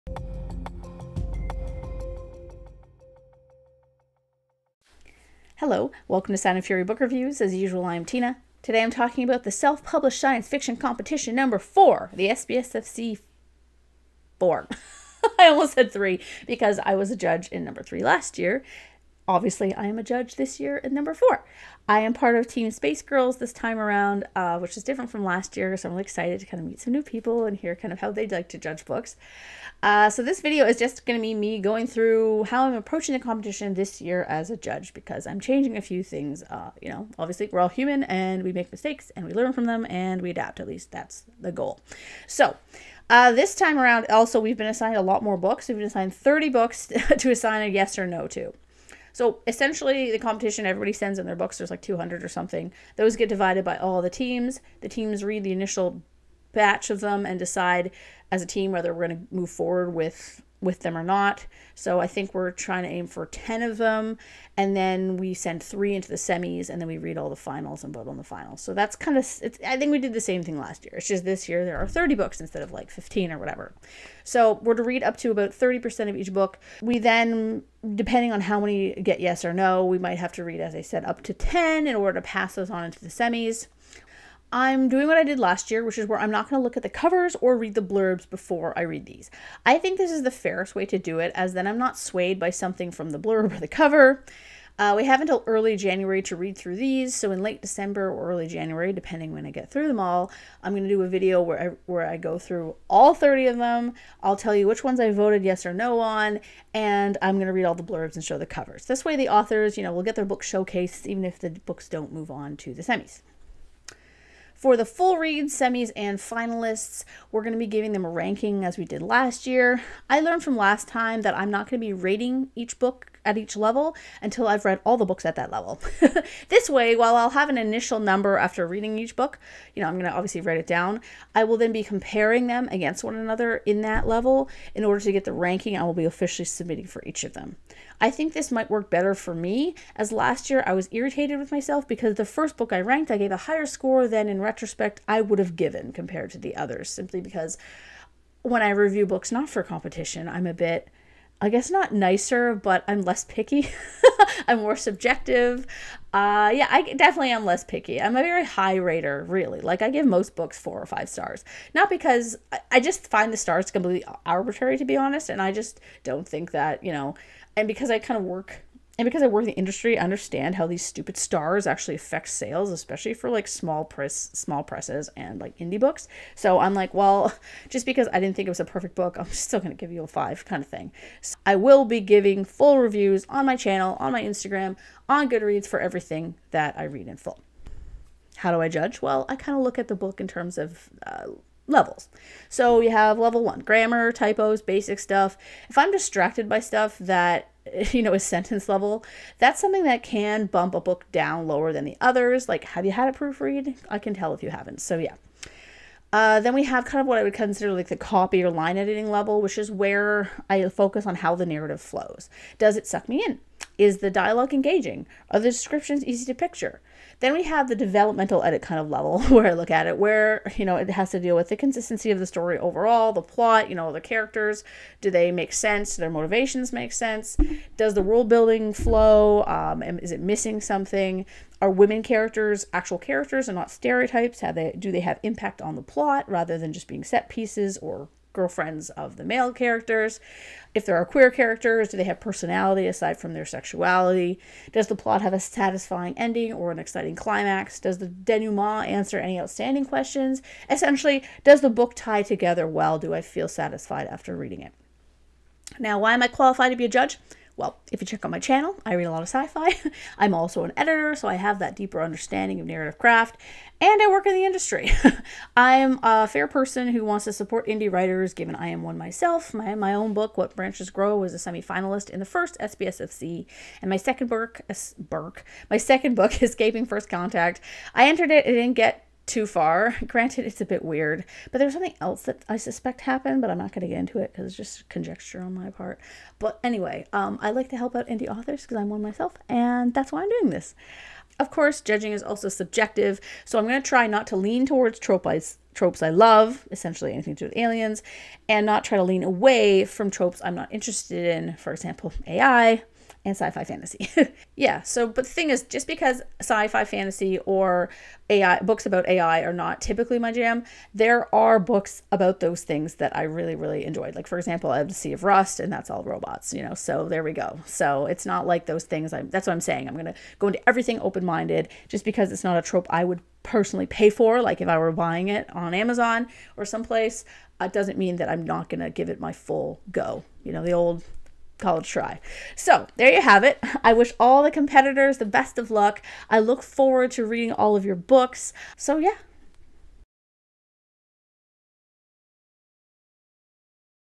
SPSFC4 - Judge Intro
I'm a judge in the Self-Published Science-Fiction Competition! Again! The video goes over the competition set-up and how I judge.

Intro Music by Longzijun, Canva for Title Card, and Shotcut for video editing. Filmed using Nexigo N60 and Maono AU-A04.